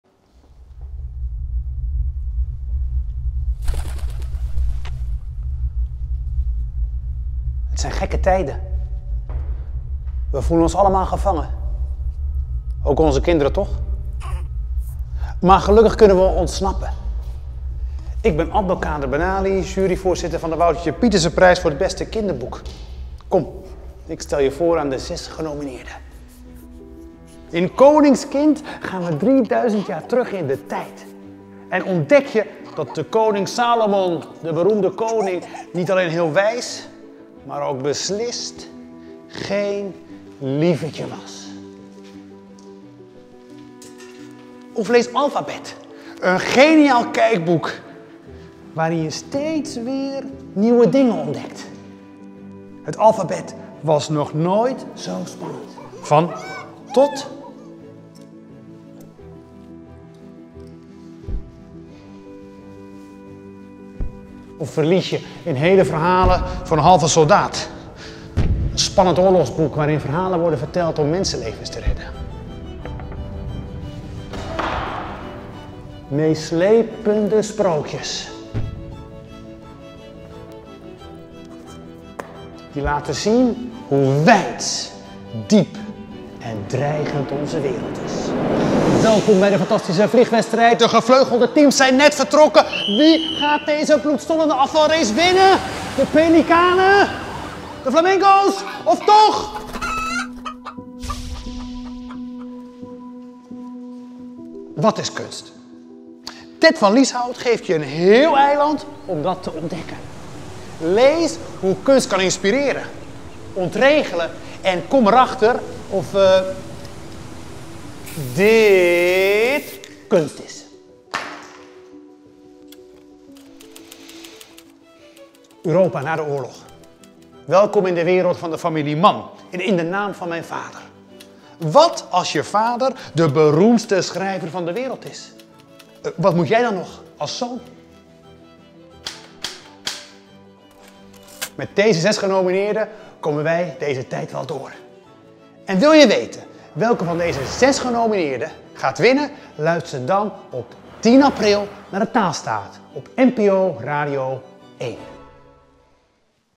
De mooiste boeken van 2020 - nominaties Woutertje Pieterse Prijs 2021
In deze video laat juryvoorzitter Abdelkader Benali je de mooiste kinderboeken van 2020 zien. Een van deze boeken maakt kans op de Woutertje Pieterse Prijs 2021. Weten welk boek gaat winnen? Luister dan op 10 april naar De Taalstaat op NPO Radio 1.